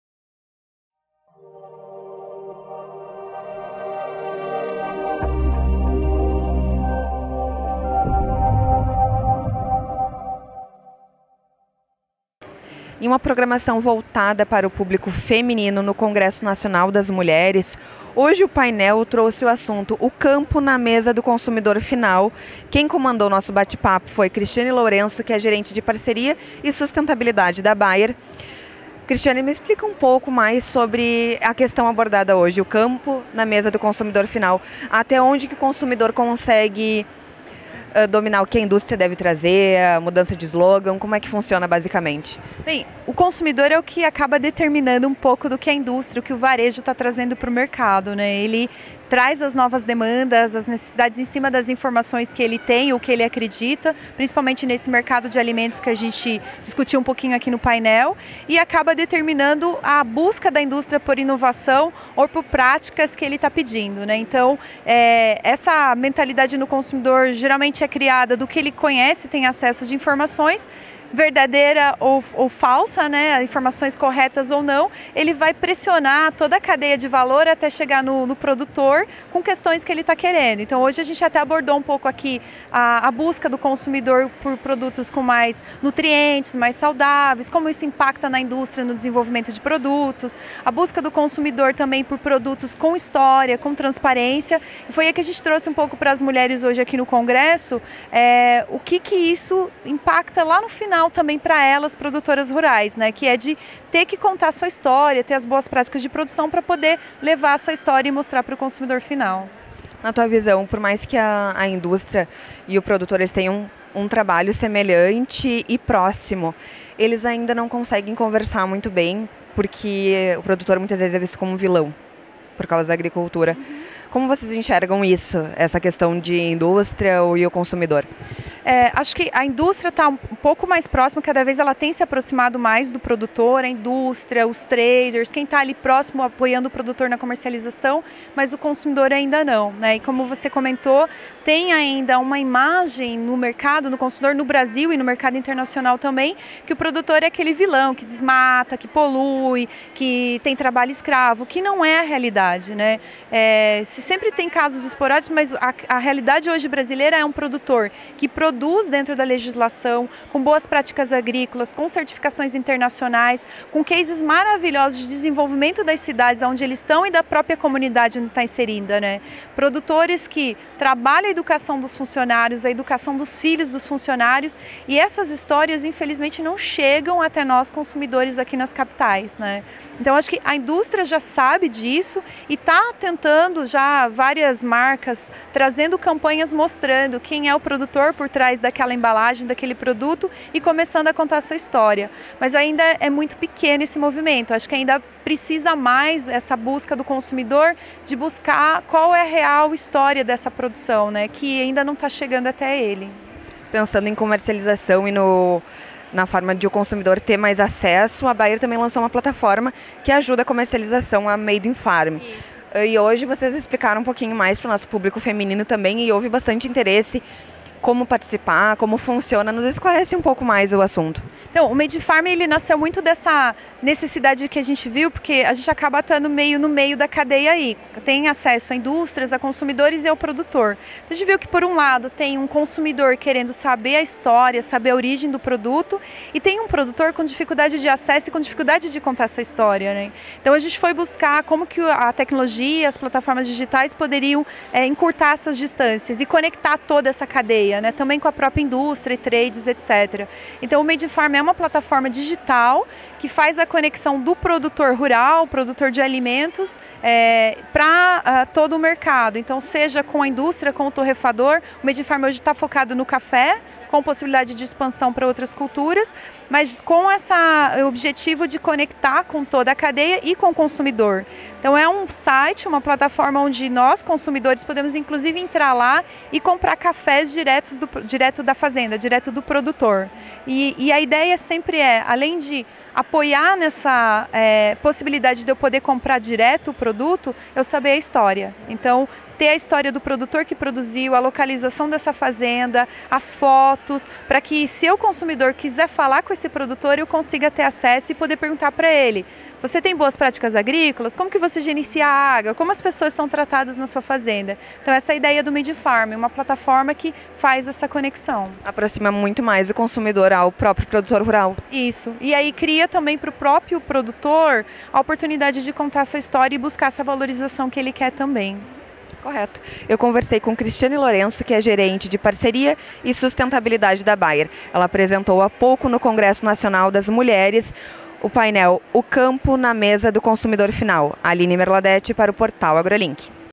O Campo na Mesa do Consumidor Final
A gerente de desenvolvimento de desenvolvimento sustentável e parcerias na cadeia de valor da Bayer, Cristiane Lourenço explicou como o consumidor determina o comportamento das industrias.